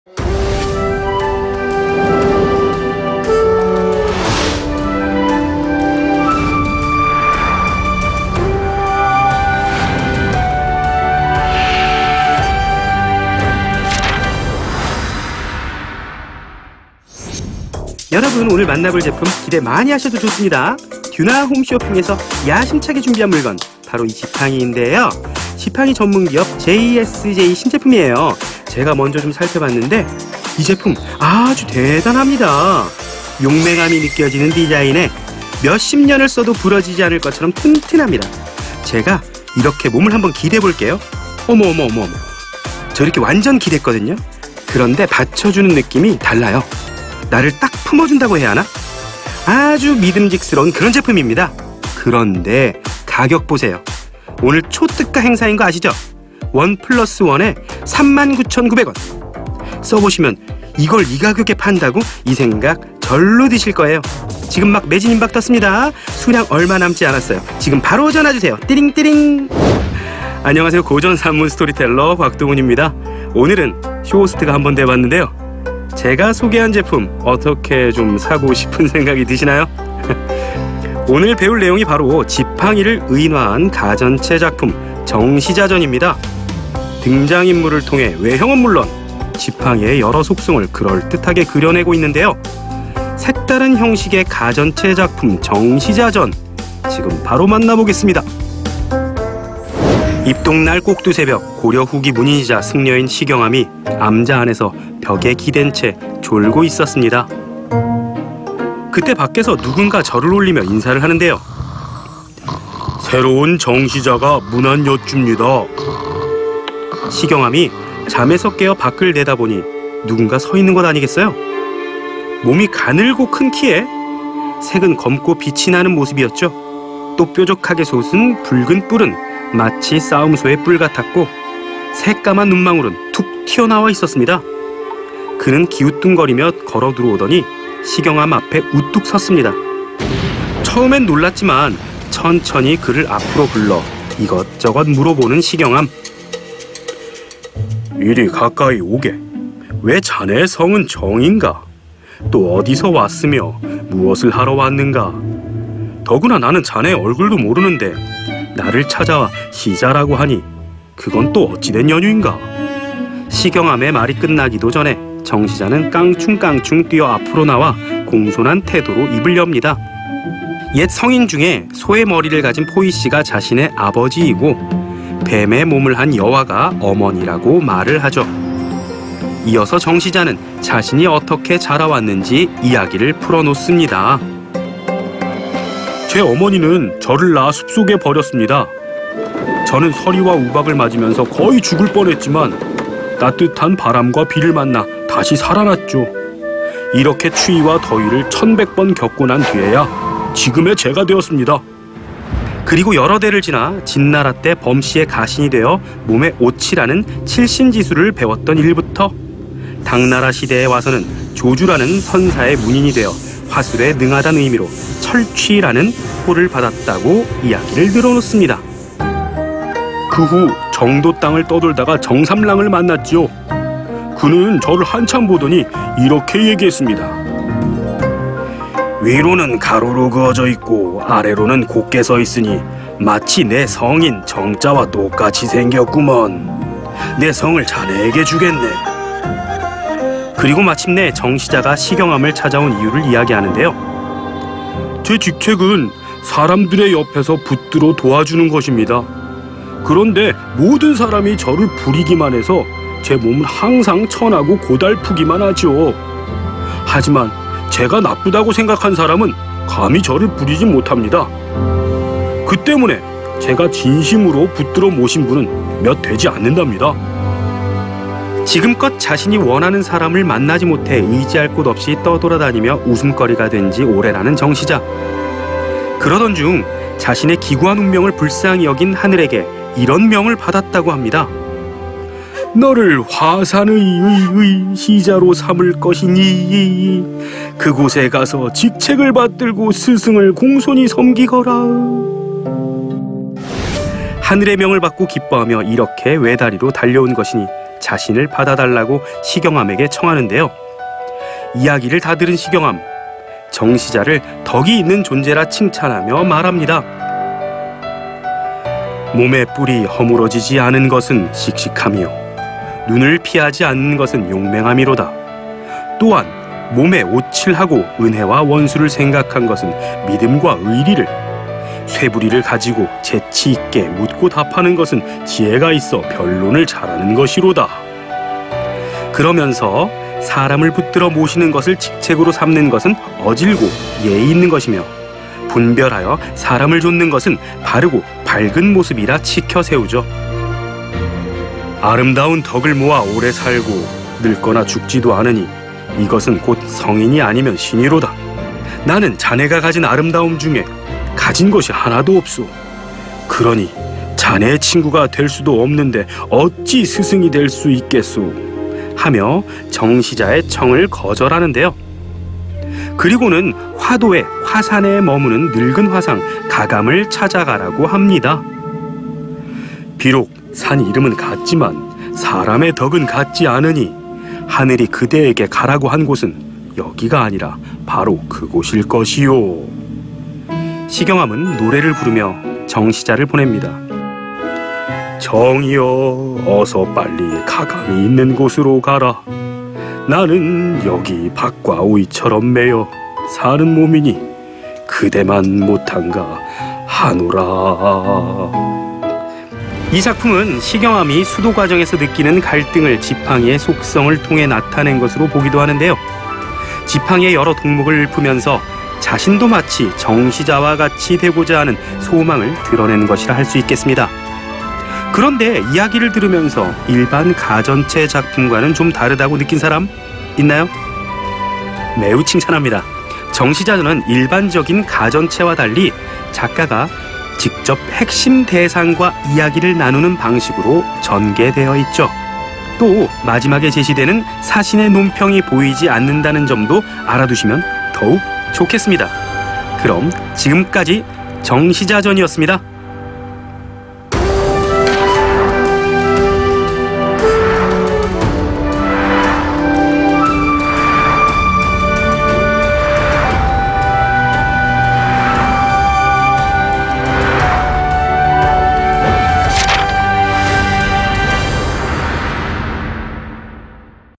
번지수 잘못 찾아오셨어요 l 정시자전 l 10 CUT 고전산문
세상에! 아직도 고전 공부에 고전한다구요?🤭
수많은 학생들에게 한 줄기 빛이 되어주던 10CUT 고전소설!
이번엔 🔥고전산문🔥이다!
아무리 읽어도 어려운 고전산문을 곽동훈 선생님과 함께 살펴보세요👀
5분 만에 어려운 고전산문 한 편 뚝딱! 할 수 있답니다📚